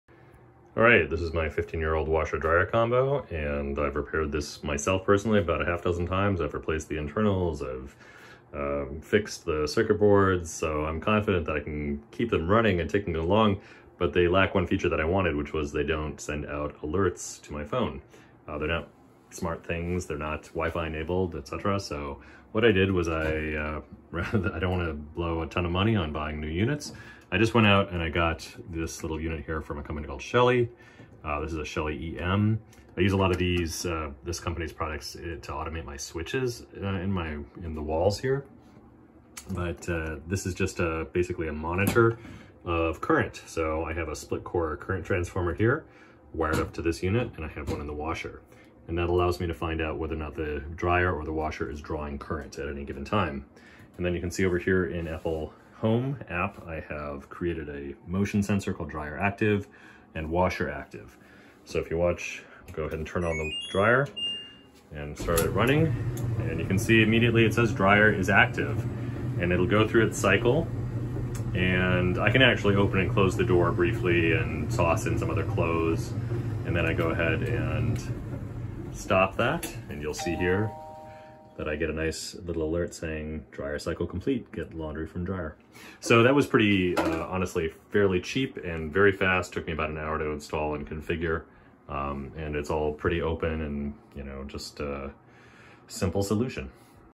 Retrofit old washer/dryer with HomeKit (Homebridge) Alerts
DIY sensors retrofitted to old washer dryer combo to let me know when laundry has to be changed. Our laundry room is in the basement and I have a light that goes on in the living room when either the washer or dryer finishes a cycle. With two growing kids we do a lot of laundry (and the kids do their own) so this gets a lot of use and is super handy.